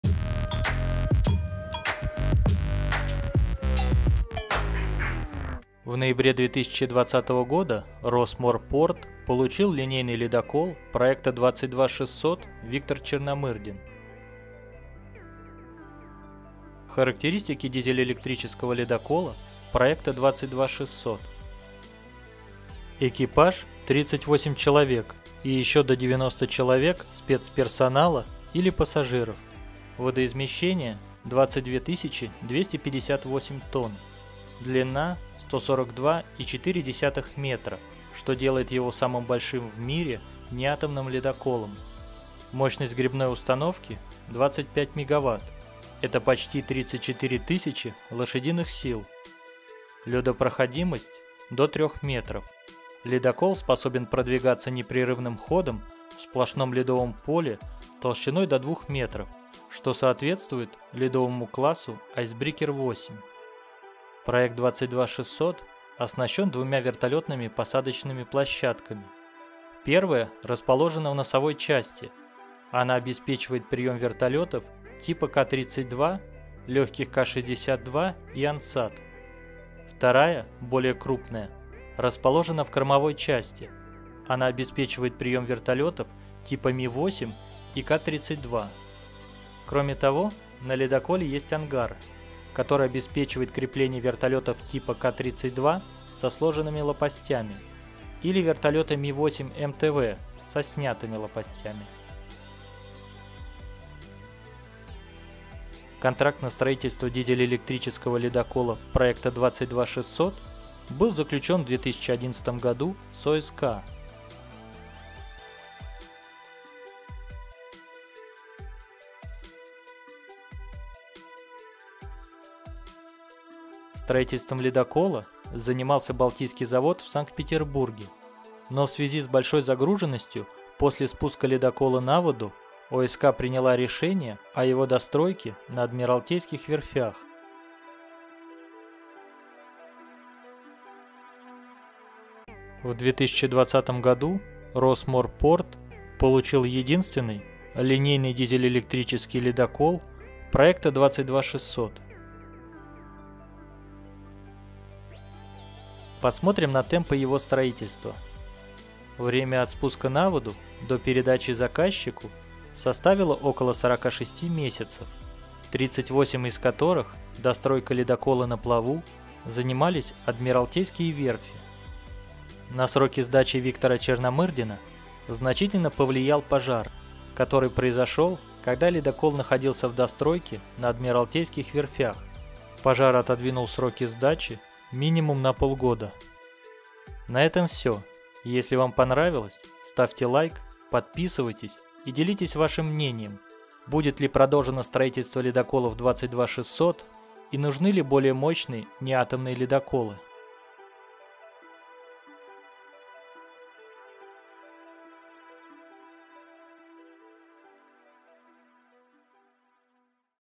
Обзор дизель-электрического ледокола проекта 22600 (ЛК-25)
Проект 22600 - это линейный дизель-электрический ледокол мощностью 25 МВт. Ледокол "Виктор Черномырдин" проекта 22600 - самый мощный дизель-электрический ледокол в России и самый большой в мире. Иногда, ледокол проекта 22600 называют ЛК-25, что расшифровывается как ЛедоКол мощностью 25 МВт.

В этом видео:
00:00 начало
00:16 характеристики
01:44 контракты
02:00 судостроительные заводы
02:21 представители проекта
02:35 темпы строительства
03:07 это интересно

Ссылки
Характеристики: